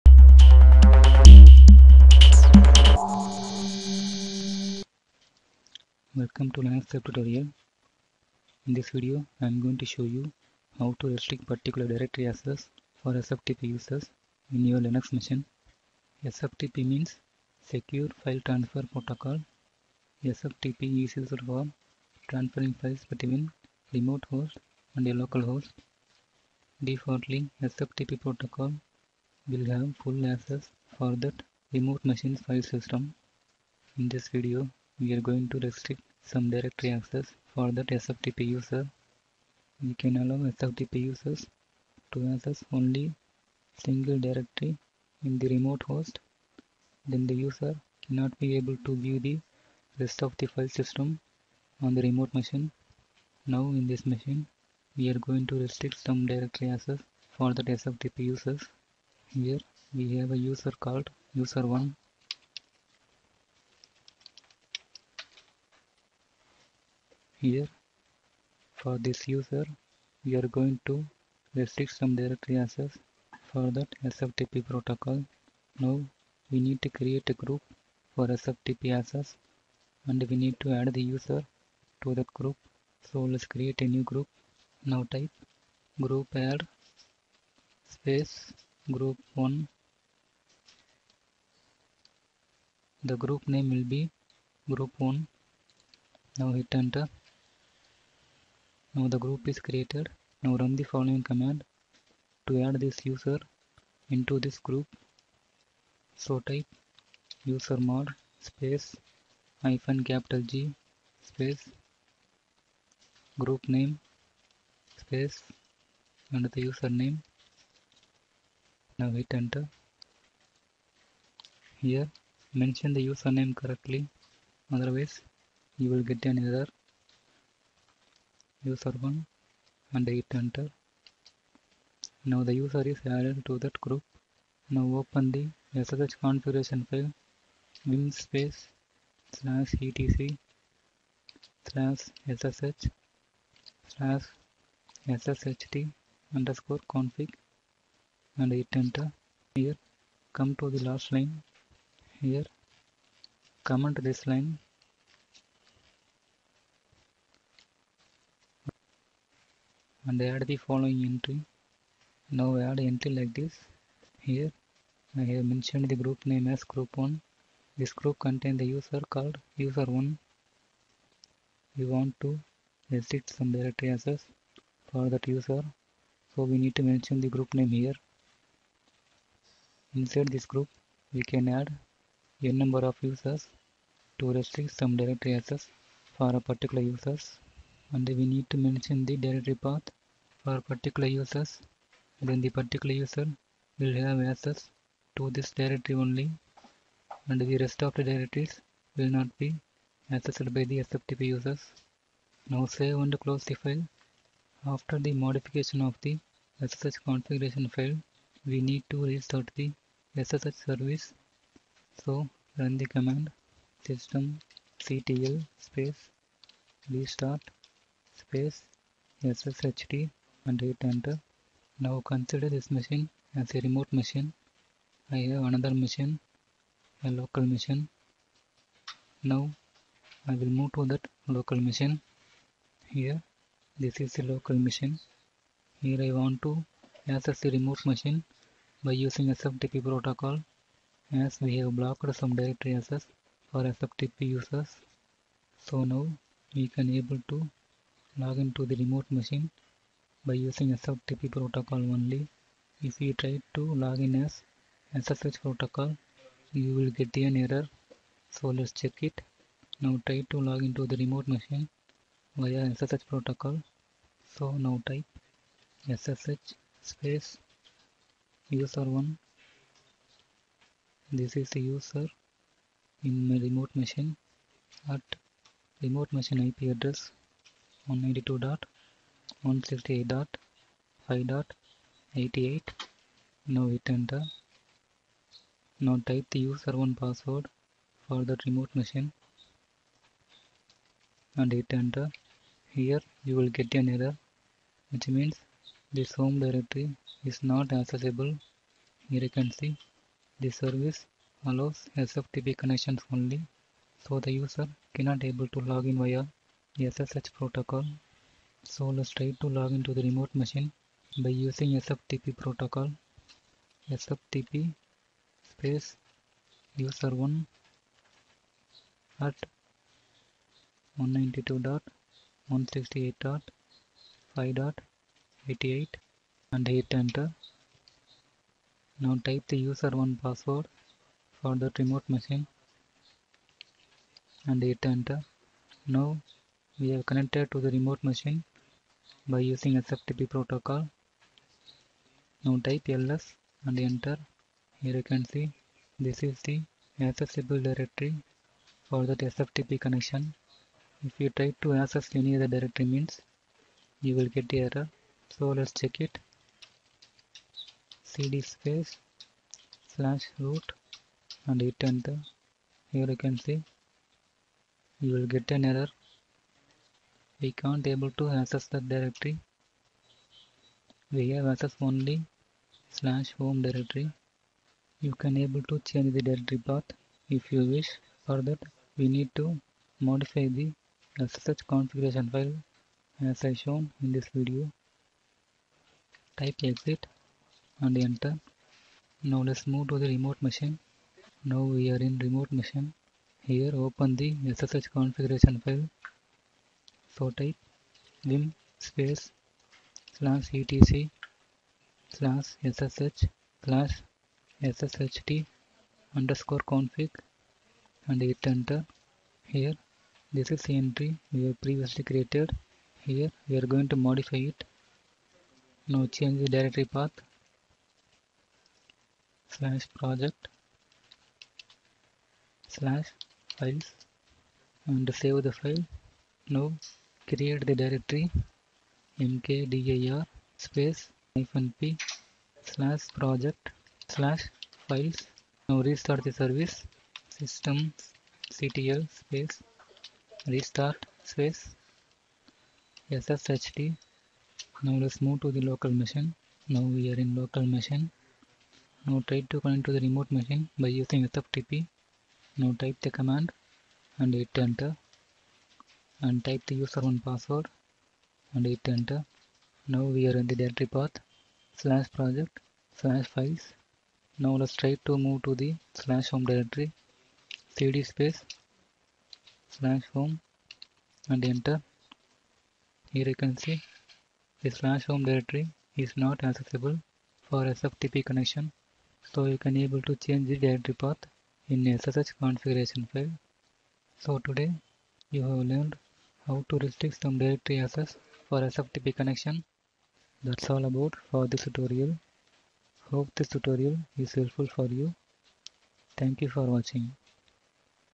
How To Restrict SFTP Users Home Directories in Linux
This video explains how to restrict SFTP users home directories in a shared server environment, to avoid unauthorized user to access other user’s files.